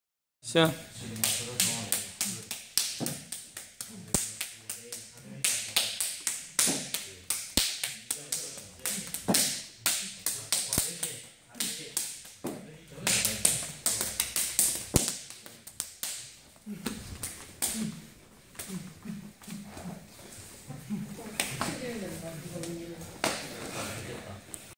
KALI Busan branch practitioners are practicing drills MTH
레벨2 테스트 준비중인 대학부와 고등부 수련생